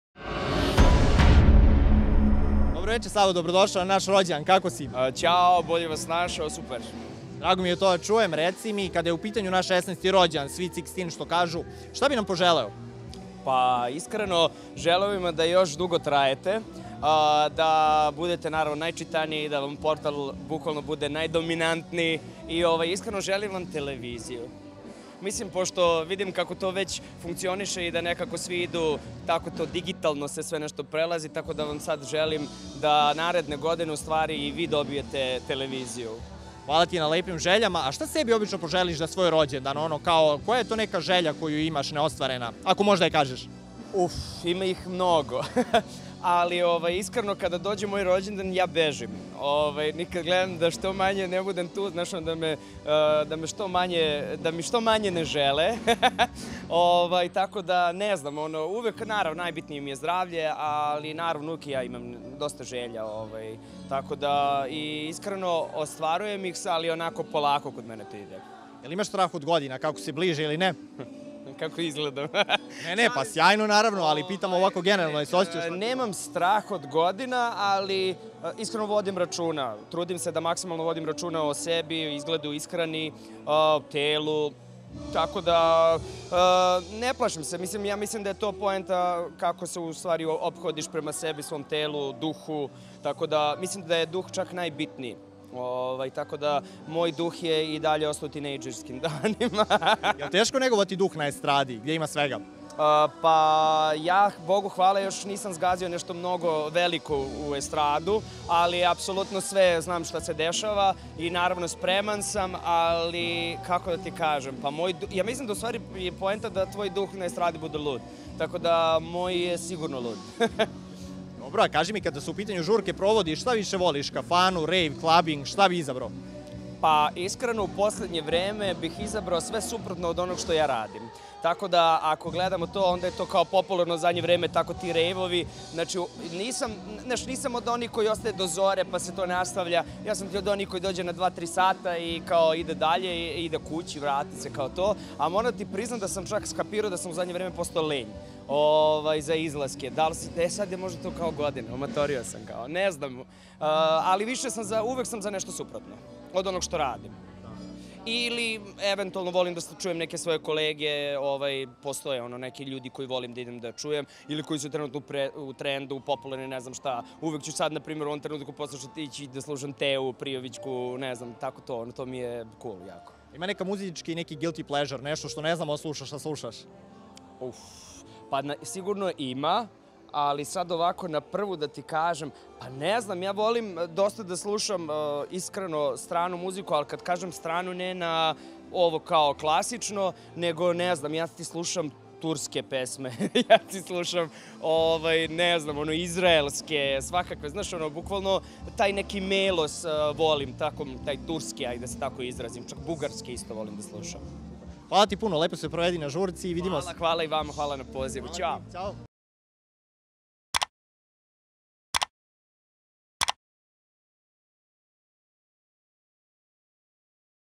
Savo Perović na Alo! rođendanskoj žurki otkrio svoje neostvarene želje
Novinar: Milan Đuričić
Snimatelj: Dušan Adamović
Montaža: Nenad Krpešić